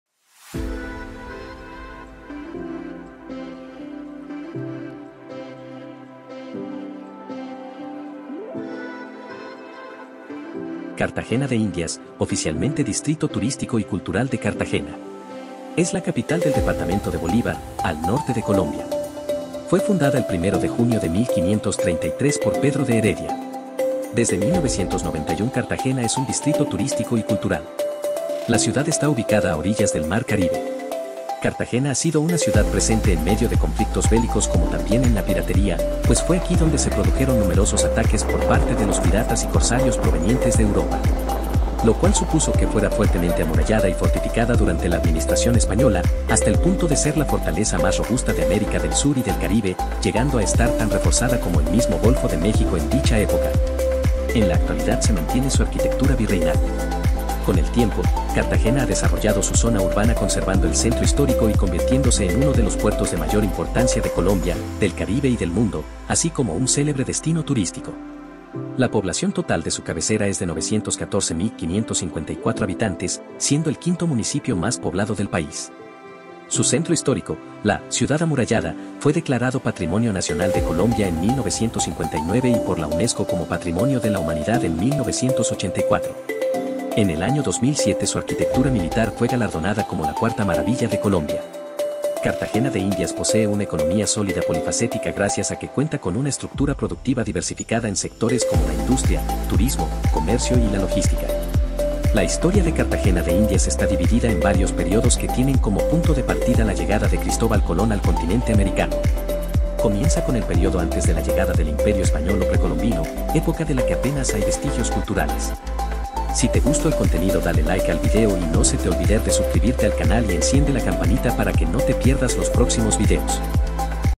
CO Cartagena de Indias la Historia! ¡Descubre como Impulsó el Futuro!
Cartagena de Indias, oficialmente Distrito Turístico y Cultural de Cartagena.

es la capital del departamento de Bolívar, al norte de Colombia

Fue fundada el 1 de junio de 1533 por Pedro de Heredia. Desde 1991 Cartagena es un Distrito Turístico y Cultural. La ciudad está ubicada a orillas del mar Caribe.

Cartagena ha sido una ciudad presente en medio de conflictos bélicos como también en la piratería, pues fue aquí donde se produjeron numerosos ataques por parte de los piratas y corsarios provenientes de Europa.